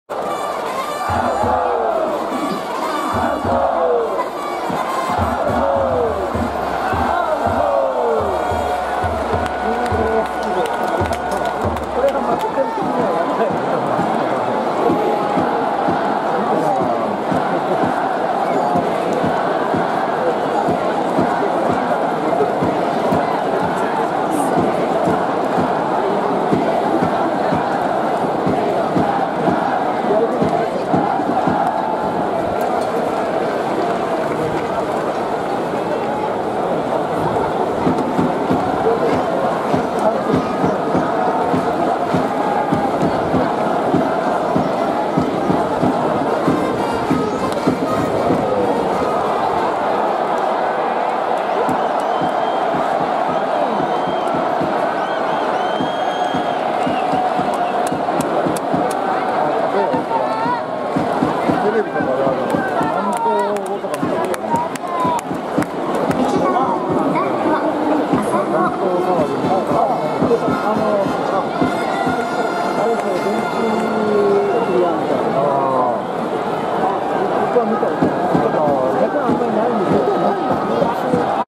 【DB-G】ピッチャー北村拓己に打順が回るも死球で変な空気になる【2023.9.2】
2023年9月2日 横浜スタジアム
横浜DeNAベイスターズvs読売ジャイアンツ（巨人）